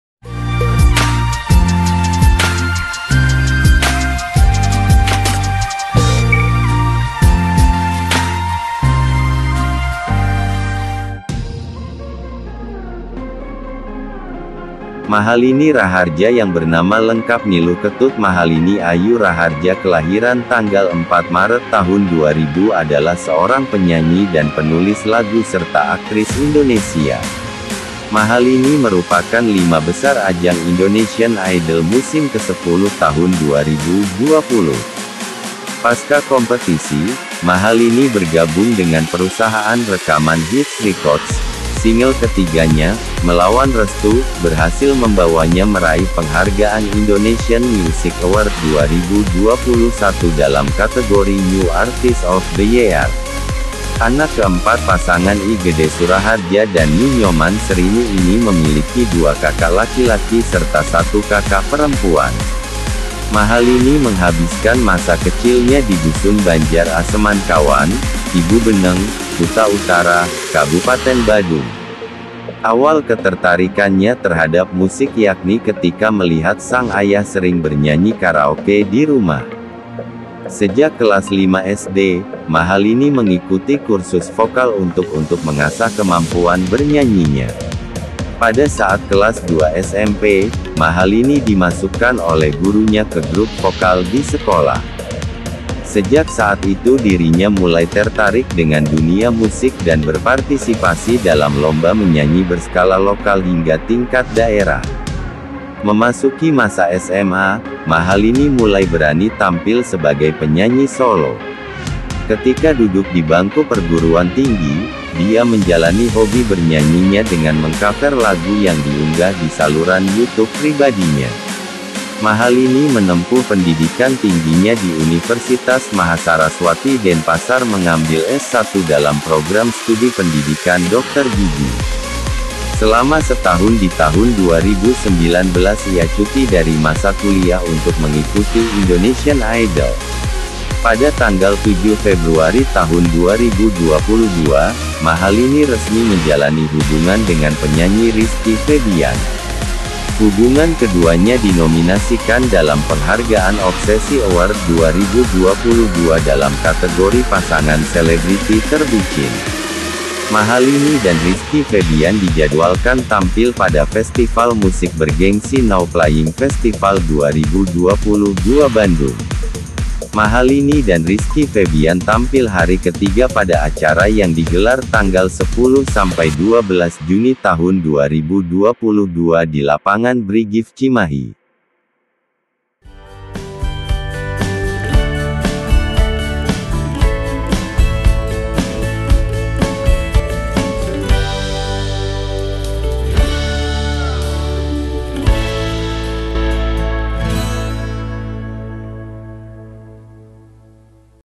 Biodata Mahalini Raharja | Profil Penyanyi Cantik asal Indonesia
Biografi Profil Biodata Mahalini Raharja Pacar Rizky Febian yang tampil pada Now Playing Festival 2022 Bandung di Lapangan Brigif Kota Cimahi 10-12 Juni 2022. Jadwal Mahalini tampil hari ke 3 di acara Now Playing Festival Bandung 2022. Mahalini Raharja Instagram IG Rizky Febian Wikipedia